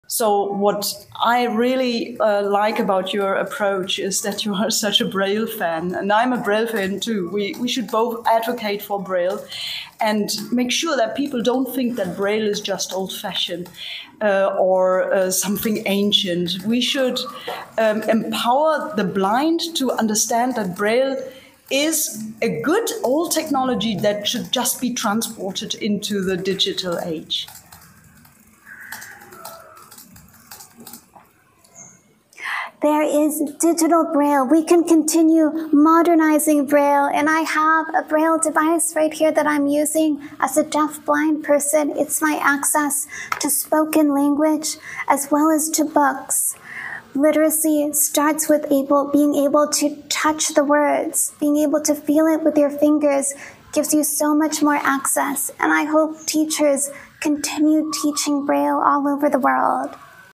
World Braille Day 2024
It’s World Braille Day! A blind teacher named Louis Braille created this tactile reading system, and now his birthday, January 4, is a day to celebrate this marvelous way to read!

Braille exists in multiple languages, and I recently had the honor of meeting Sabriye Tenberken, also blind, who invented and taught Tibetan Braille.

In the age of audiobooks and AI, do you think Braille reading will die?

Descriptive Transcript:

(Sabriye and Haben are sitting next to each other in an auditorium, a sign behind them says, “Kanthari.” Sabriye is a white woman in her fifties with a German accent, and Haben is a Black woman in her thirties with an American accent. As Sabriye speaks, Haben is reading her words on a Braille display.)

Sabriye: So, what I really like about your approach is that you are such a Braille fan and I'm a Braille fan, too. We should both advocate for Braille and make sure that people don't think that Braille is just old-fashioned, or something ancient. We should empower the blind to understand that Braille is a good old technology that should just be transported into the digital age.

(Typing sounds. Haben reads on the Braille display.)

Haben: There is digital Braille. We can continue modernizing Braille, and I have a Braille device right here that I'm using as a Deafblind person. It's my access to spoken language as well as to books. Literacy starts with being able to touch the words. Being able to feel it with your fingers gives you so much more access, and I hope teachers continue teaching Braille all over the world.